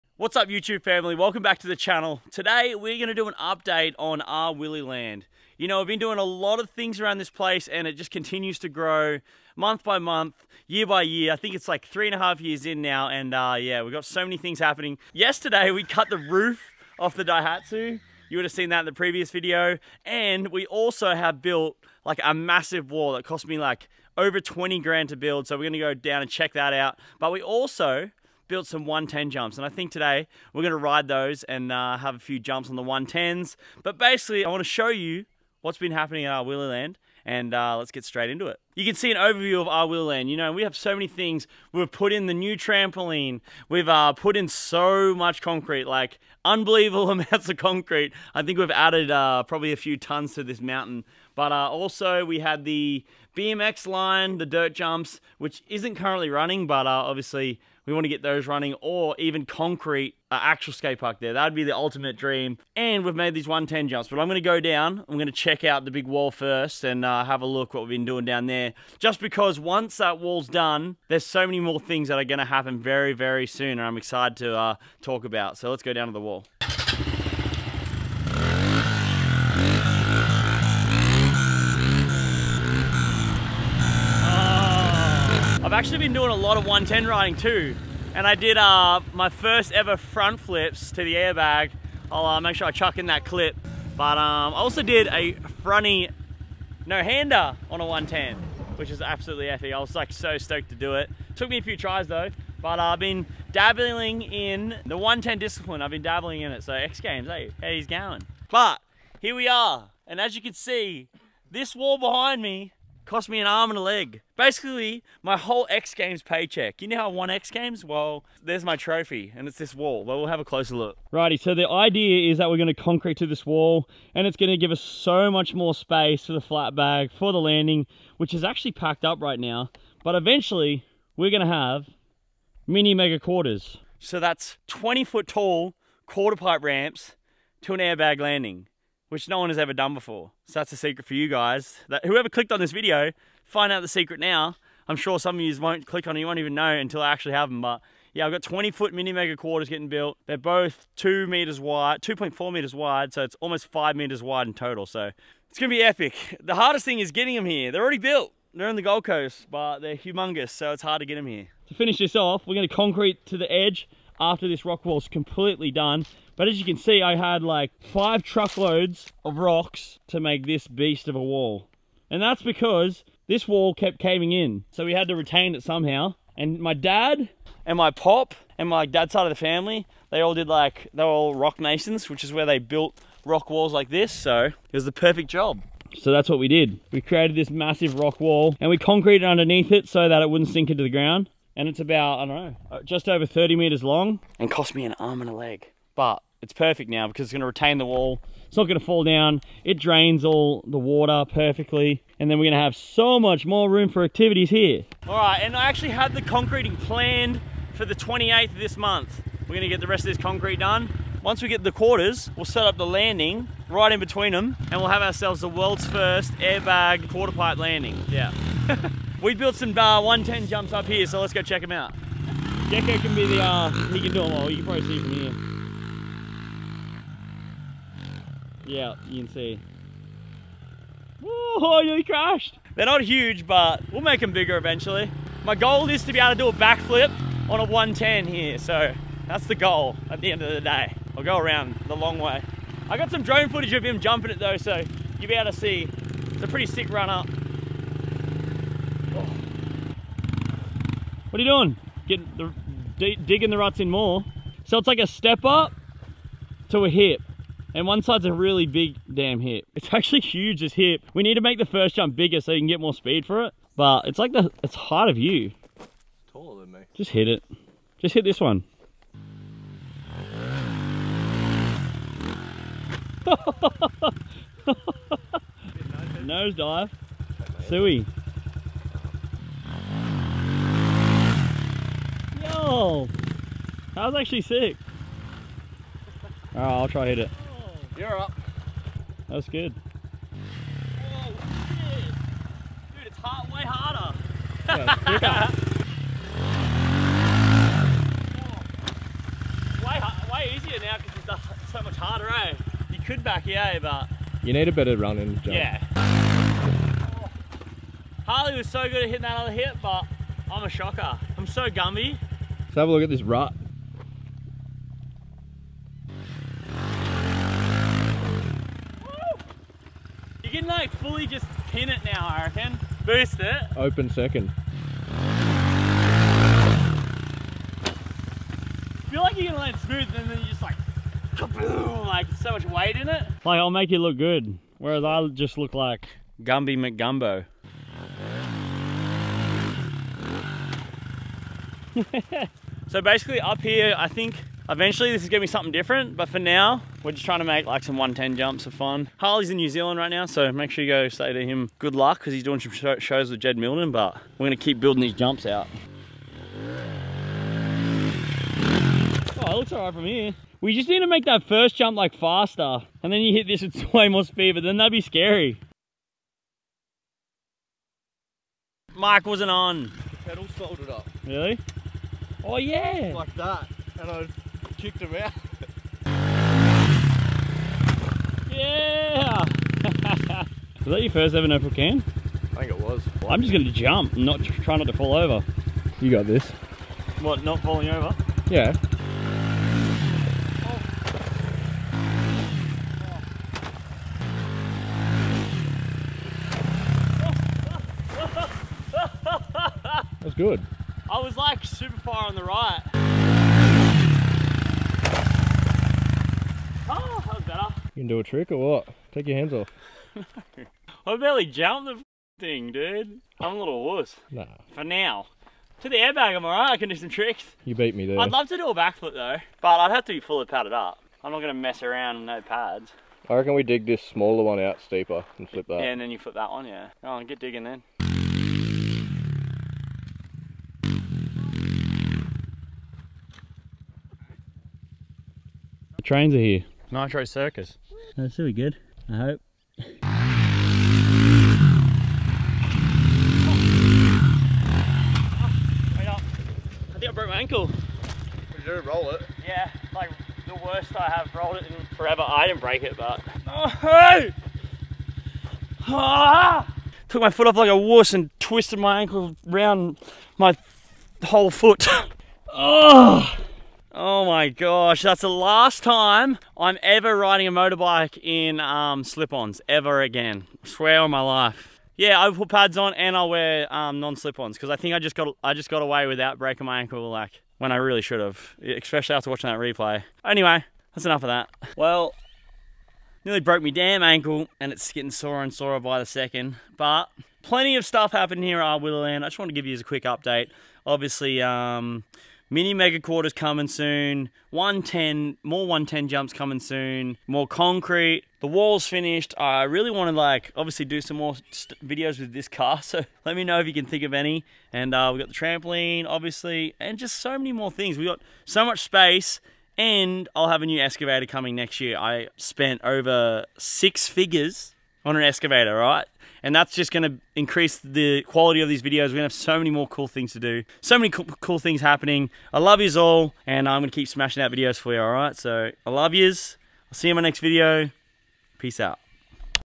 DESTROYED MY ANKLE ON PIT BIKE | RWILLY LAND UPDATE #6
Time for another update on progress at RWilly Land. We have done some upgrades and have some amazing future plans so I wanted to run through some of them with you all. We also built a 110 pit bike jump and I ended up destroying my ankle trying to jump it!